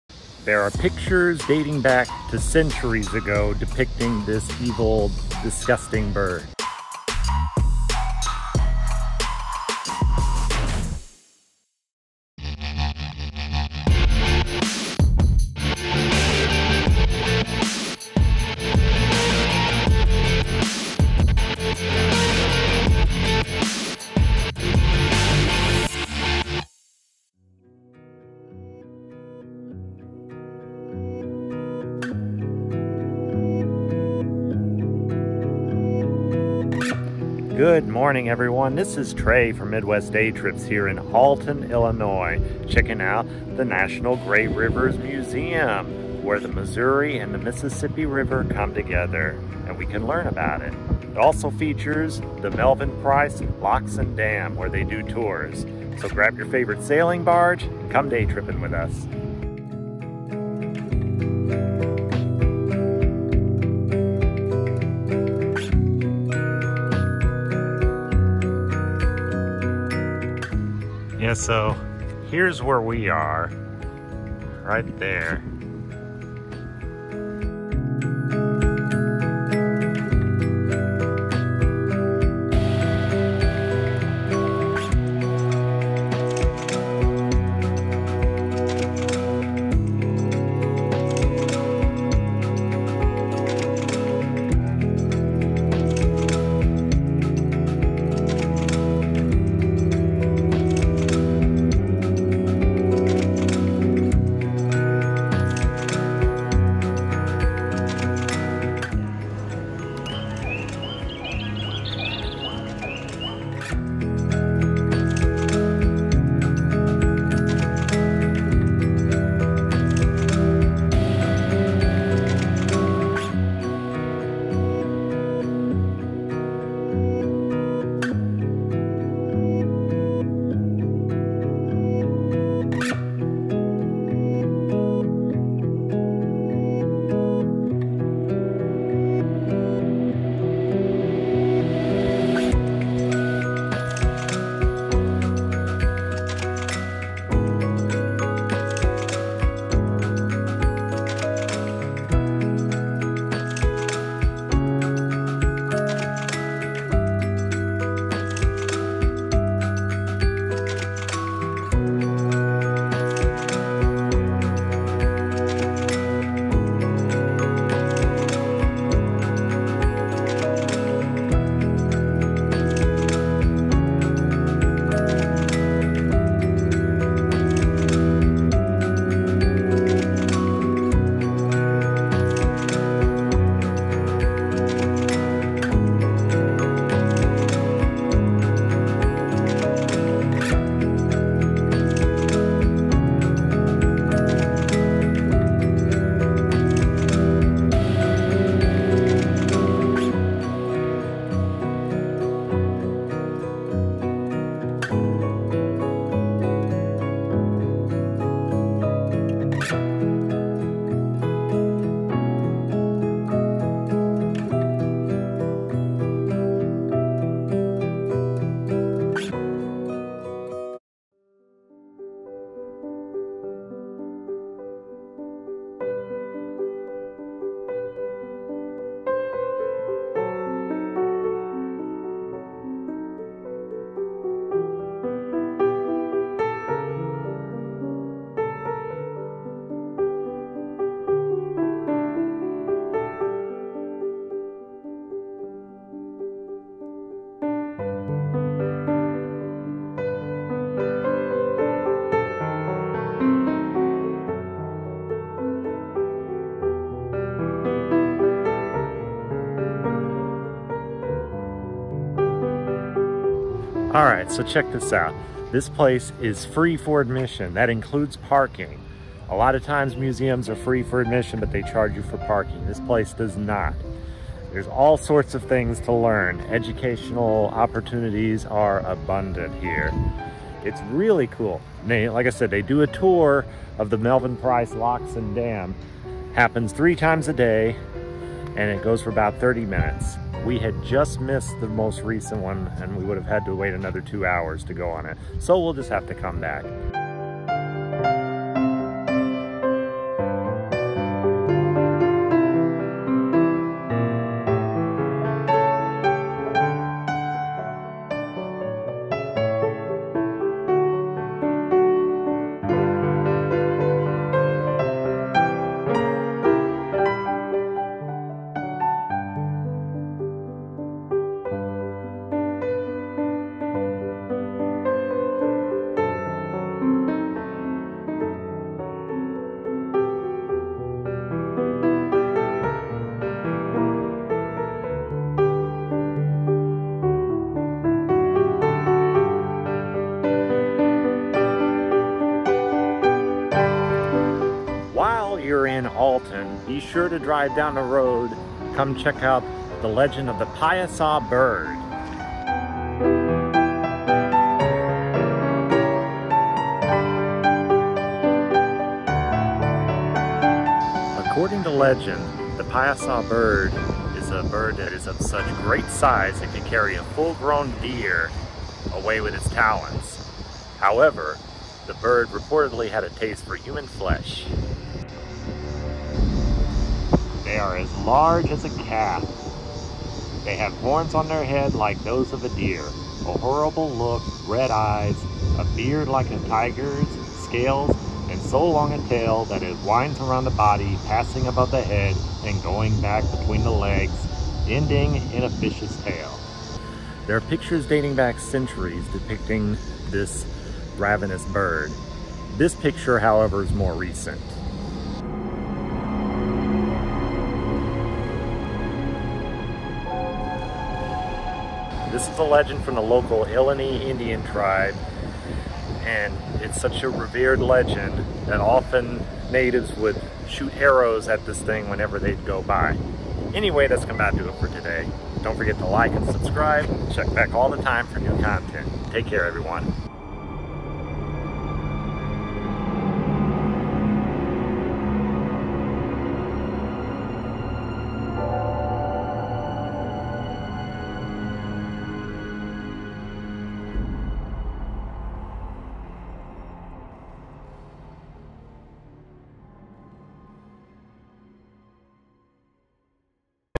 Great Rivers & Man Eating Birds - National Great Rivers Museum - Alton, IL
Join us as we check out The National Great Rivers Museum in Alton, IL, where the Mississippi River and the Missouri River come together.

We also check out the legend of the Piasa Bird, a mythical creature who was believed to have a taste for human flesh!